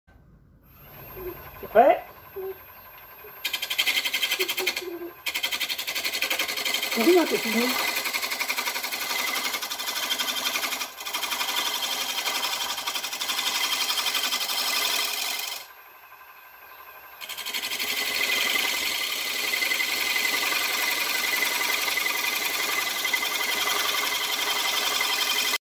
Fantin, 5 ans et déjà au tour à bois
Il n'est jamais trop tôt pour commencer à  faire des copeaux.